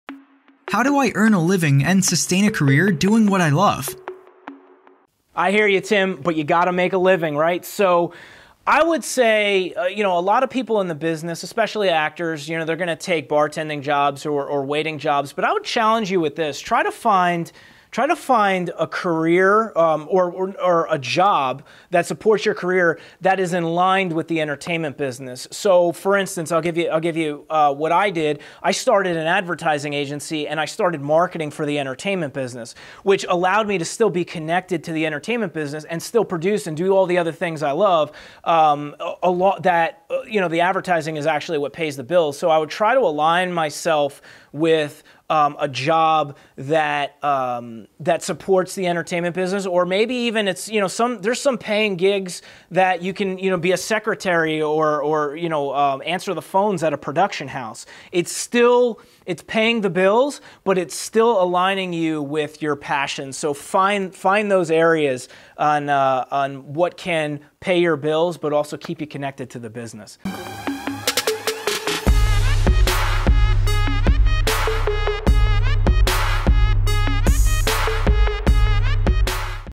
Can you earn and living while doing what you love? We say YES!
Can you earn and living while doing what you love? We say YES! Find out more below!

How do I earn a living and sustain a career doing what I love?

I hear you, Tim, but you’ve got to make a living, right? I would say a lot of people in the business, especially actors, they’re going to take bartending jobs or waiting jobs. But I would challenge you with this: try to find a job that supports your career that is in line with the entertainment business.

For instance, I’ll give you what I did. I started an advertising agency and I started marketing for the entertainment business, which allowed me to still be connected to the entertainment business and still produce and do all the other things I love. The advertising is actually what pays the bills.

So I would try to align myself with a job that supports the entertainment business. Or maybe even – there’s some paying gigs that you can be a secretary or answer the phones at a production house. It’s paying the bills, but it’s still aligning you with your passion.

So find those areas on what can pay your bills but also keep you connected to the business